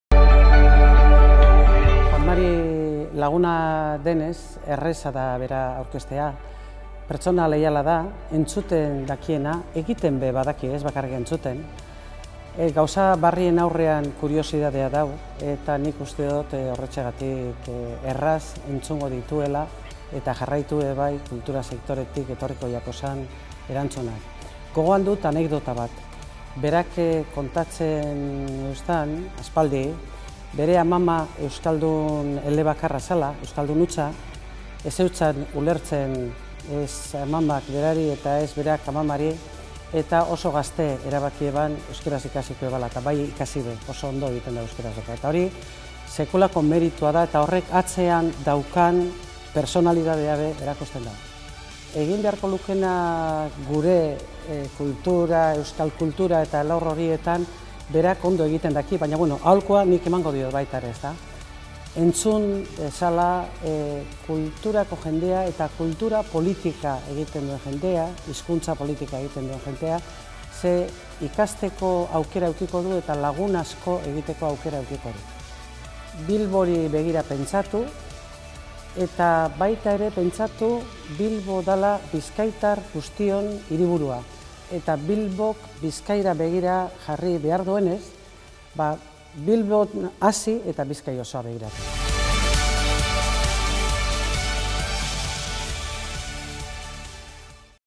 Juan Mari by Miren Josune Ariztondo
Miren Josune Ariztondok, Kultura Saila Diputatuak, Juan Mari Aburto deskribatzen digu.